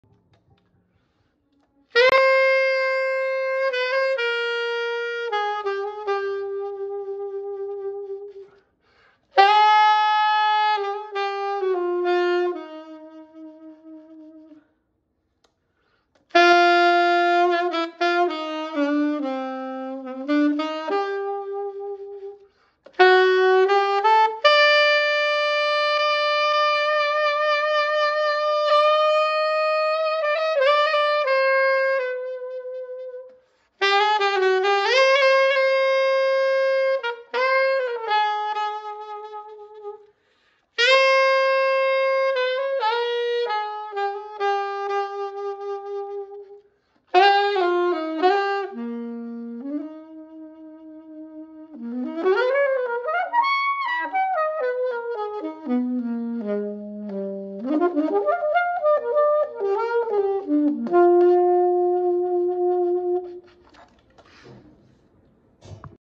Selmer Mark VI Alto Sax Original Lacquer 232xxx Serial Number With full overhaul! New York Sax shop
Hi there saxophone community! 😍🎷🙏

I must say I really love how this Late Mark VI Alto plays… it is very flexible and so easy to play in tune! 🥰🙏🎷 I think that some of these late serial number horns really play awesome! 232xxx.